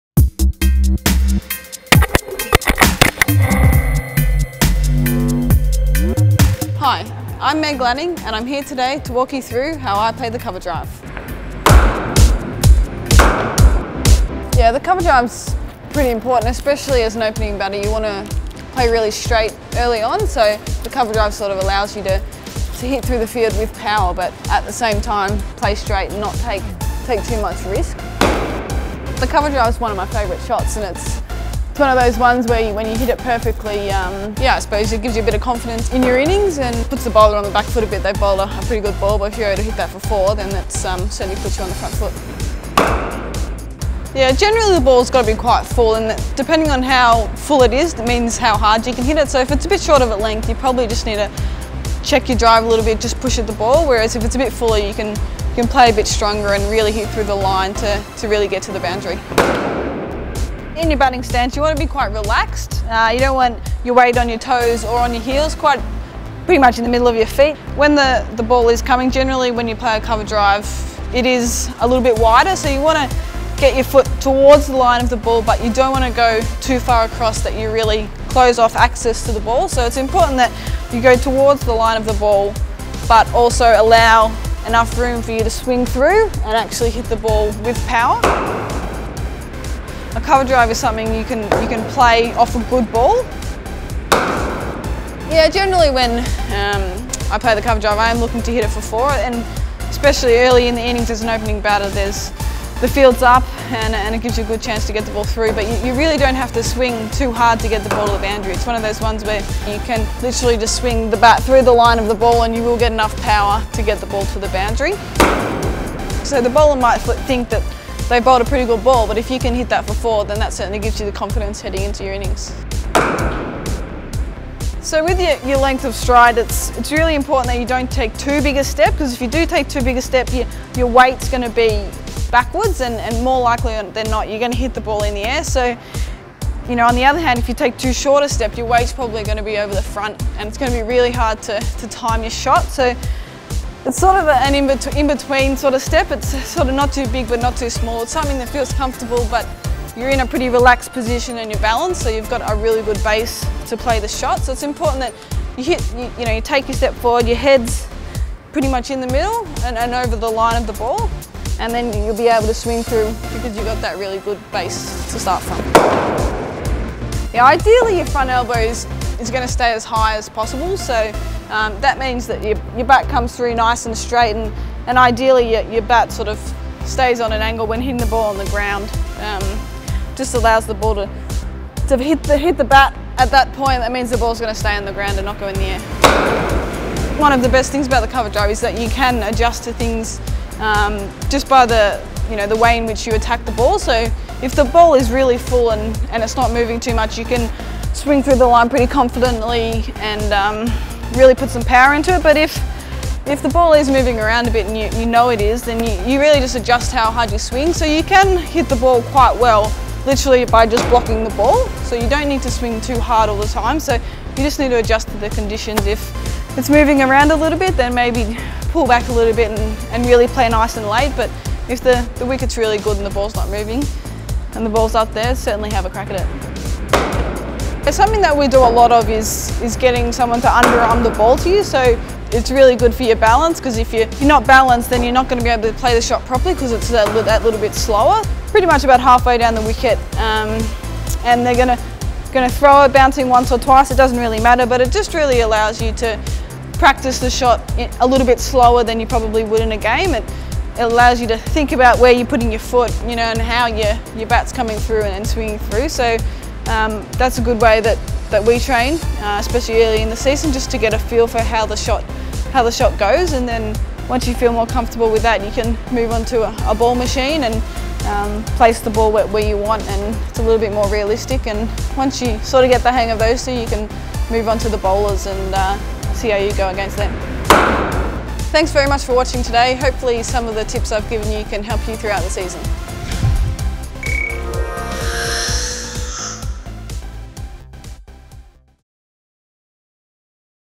Lanning Master Class: The cover drive
Commonwealth Bank Southern Stars captain Meg Lanning, who has taken all before her in the past twelve months, provides her insight into how to play the cover drive